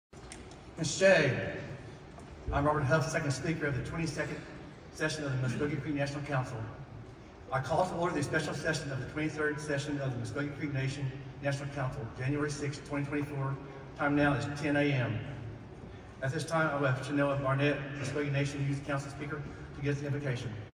7 2024 National Council Inauguration-Second Speaker Hufft Call to Order
7 2024 NC Inauguration Second-Speaker Hufft Call to Order